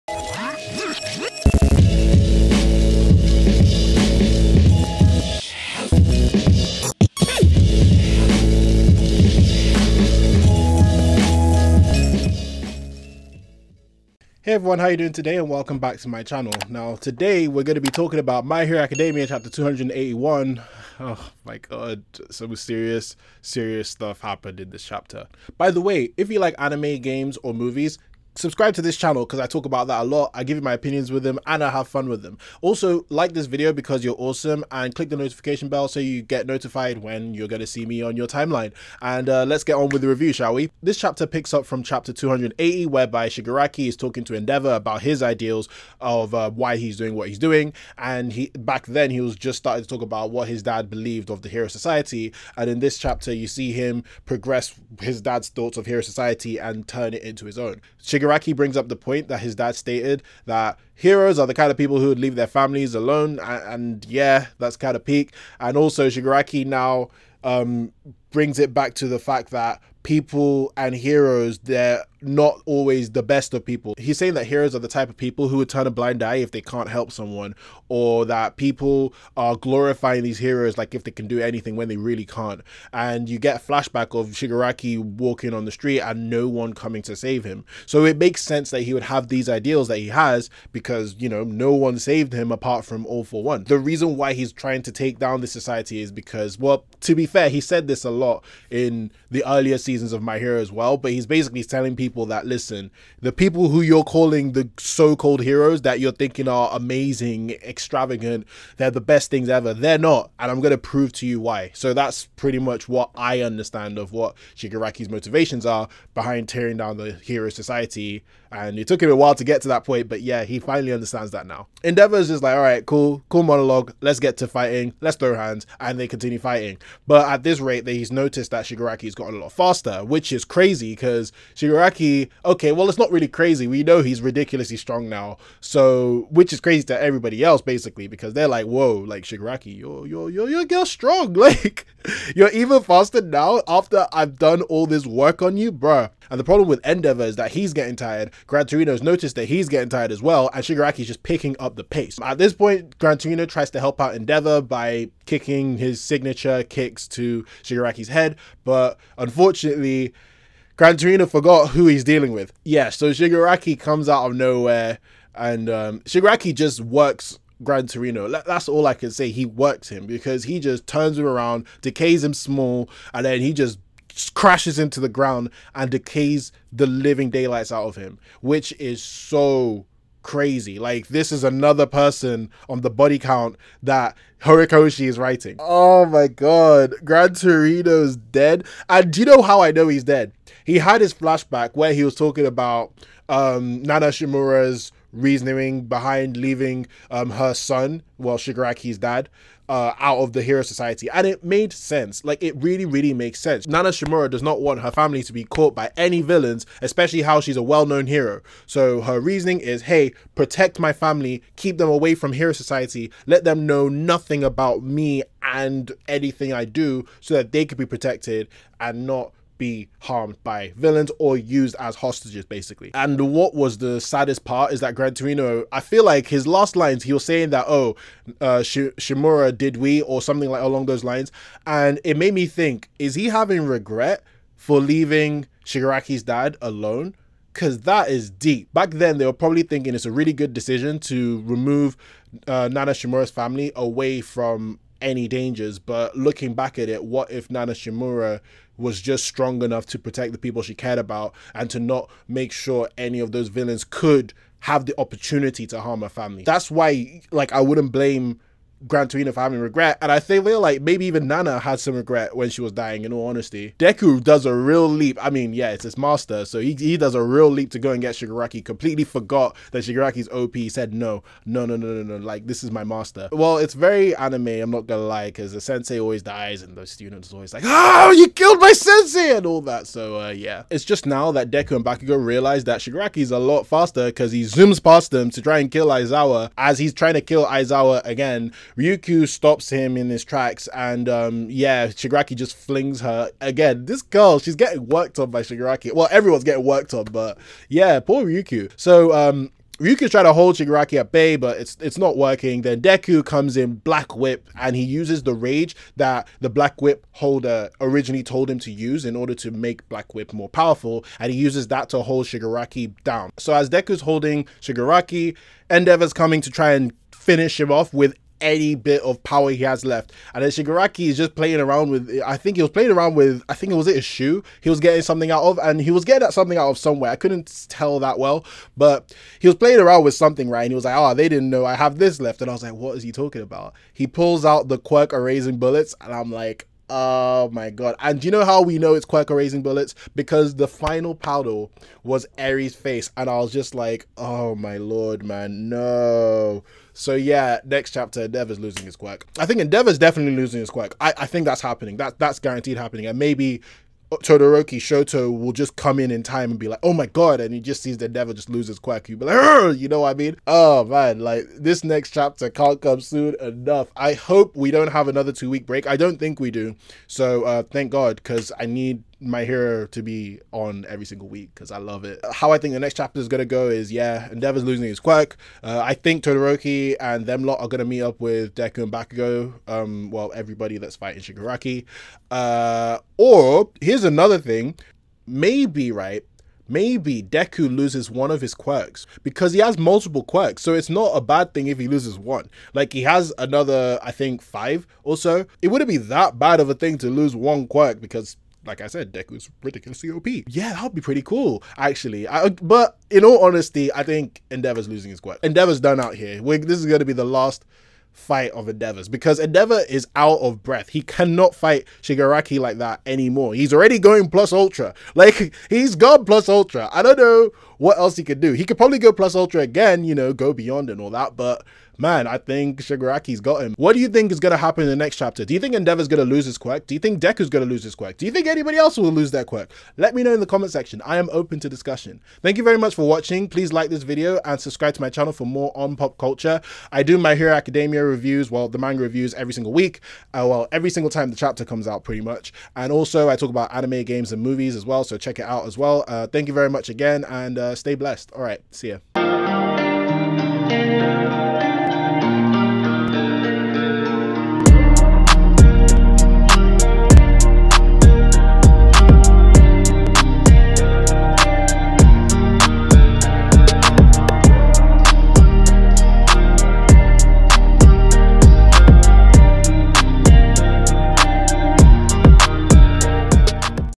MY HERO ACADEMIA CHAPTER 281 (MANGA TALK)
My Hero Academia 281 was another banger! The insane fight between Shigaraki and the rest resulted in Gran Torino having regret. MHA Chapter 281 showed that Horikoshi is not done on the death list. Will this be the end of our heroes??

Daibi Pax-Harry does My Hero Academia Manga reviews every time a new chapter comes out (usually weekly)